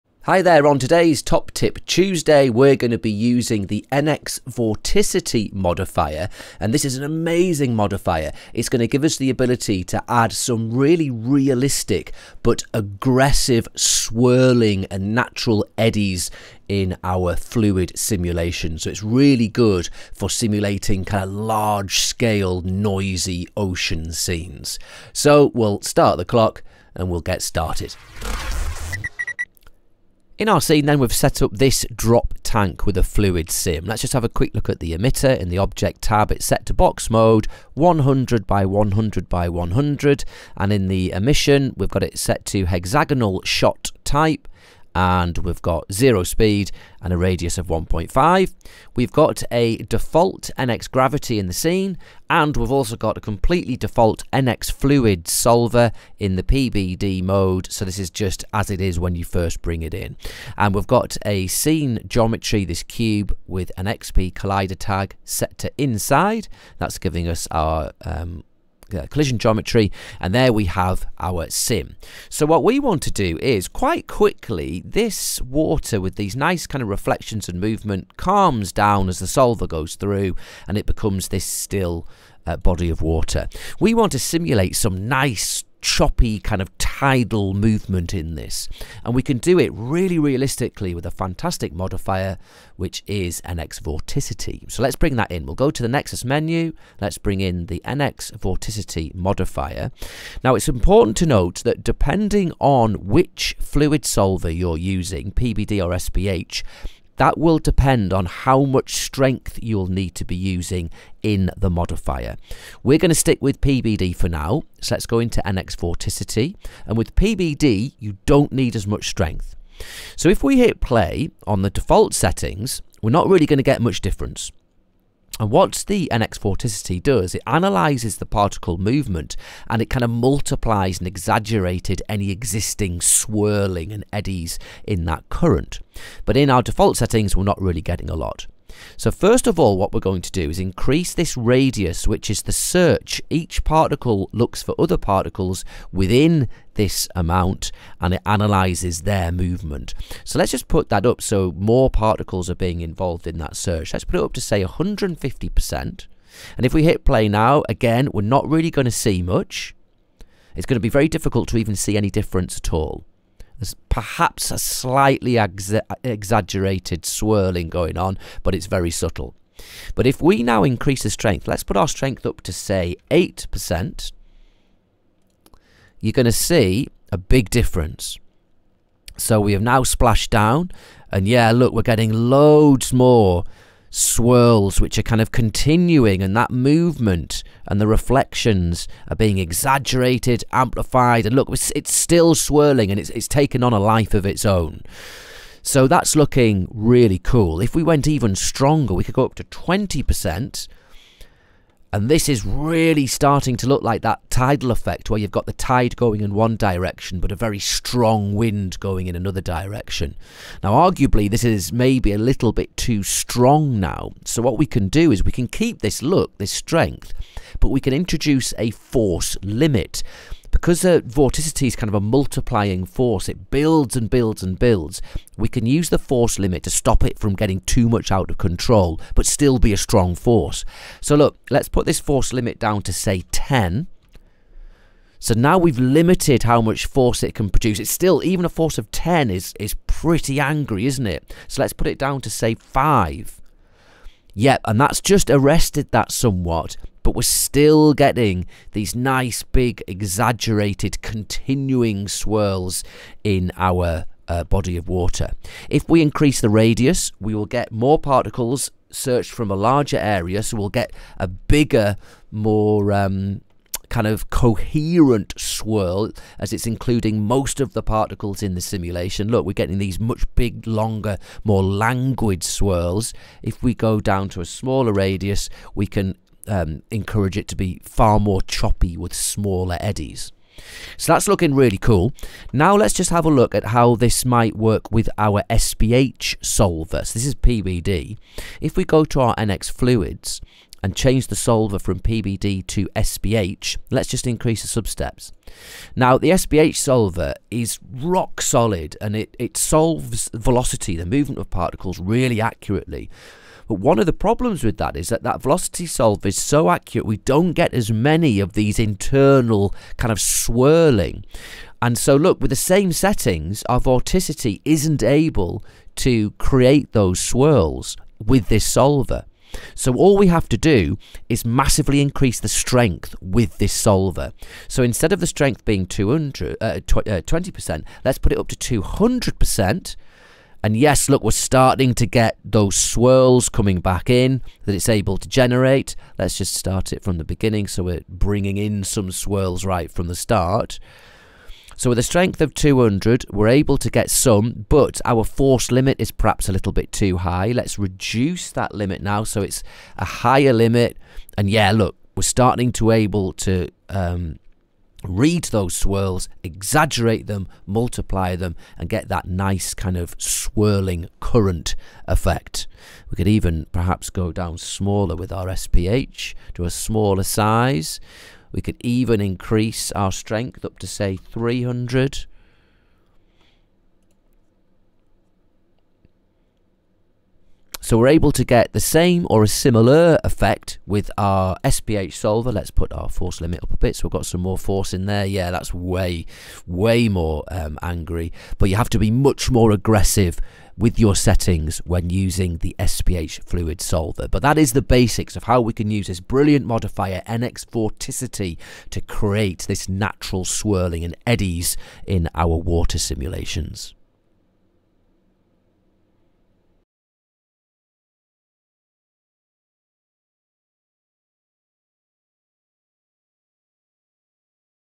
Top Tip Tuesday - nxVorticity
Today's Top Tip Tuesday highlights NeXus, as we generate agitated water effects, with naturally swirling currents and eddies. The nxVorticity settings, including radius and strength, are analyzed and explored, when used in conjunction with the PBD and SPH fluid solvers.